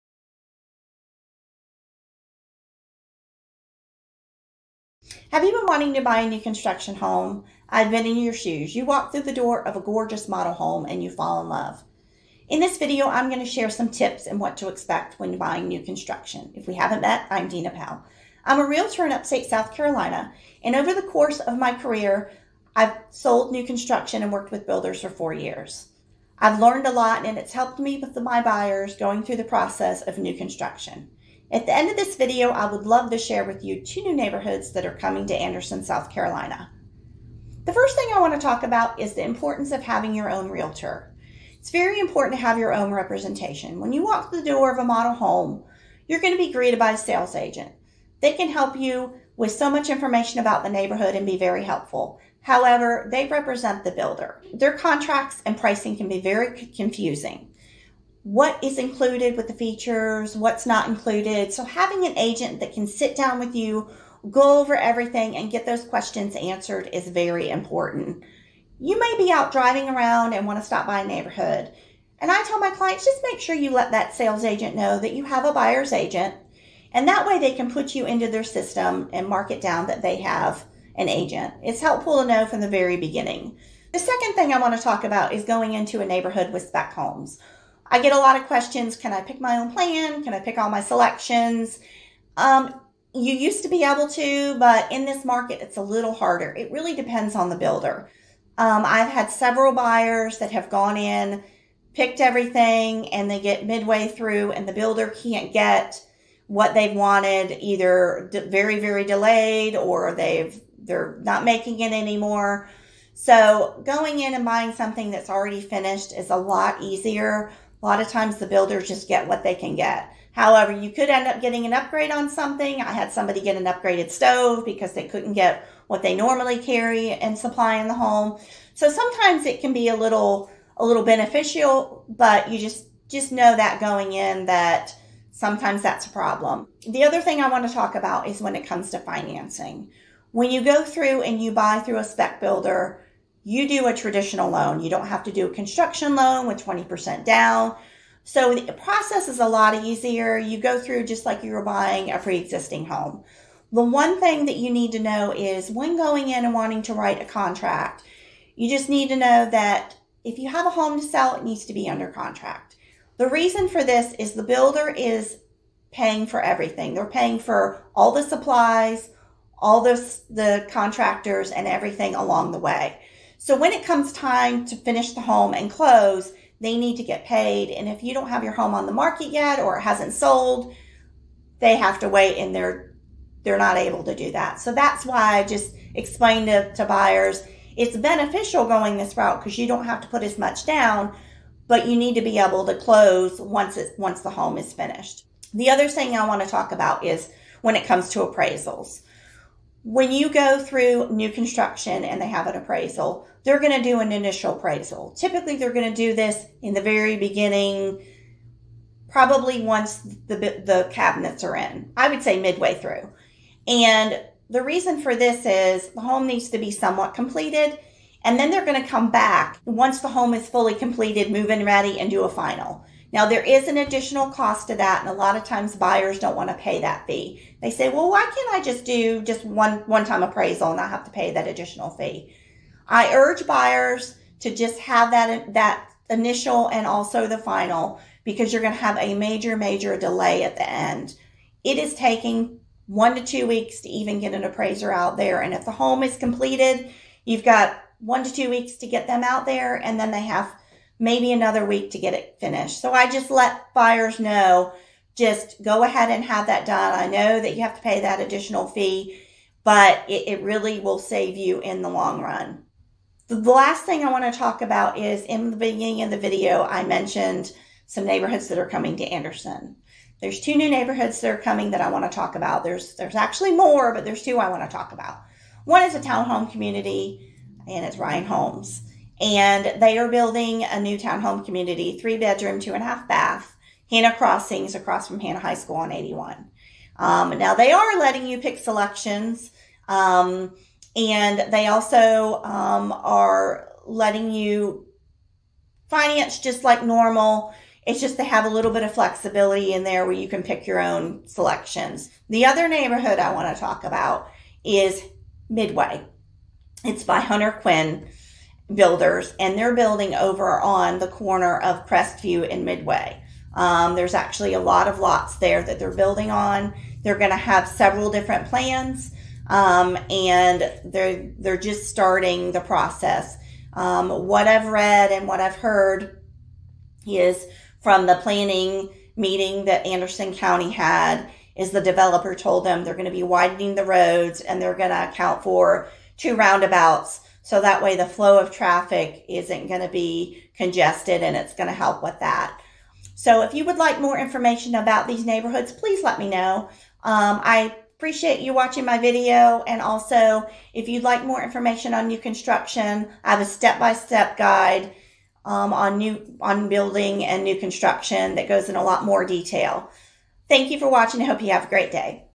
What to Know When Buying a New Construction Home
Discover details on new construction homes as well as a sneak peek into 2 new neighborhoods coming to Anderson SC!